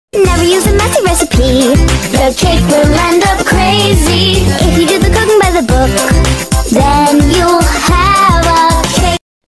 never use a messy recipe! // ( ⚠️ : TW + my au [more in desc] // GHOST and Pals
Honestly don't ask how i even came up with such a bad video idea.

Anyways Uhm HERE. is a sum up of this bit of my COMMUNICATIONS case one au.

- 3 years into Nancy and Henry's marriage [1953], Henry tried to leave Nancy and she killed him. she kept his body in her basement for around 5 months, telling everyone he'd been on a business trip when she was asked about his absence. THEN Uh. Nancy caved finally and pretended her husband had fallen ill and passed away and she killed herself soon after realising her husband was actually dead cuz she'd been gaslighting herself gaslight gatekeep girlboss👍👍👍(😐)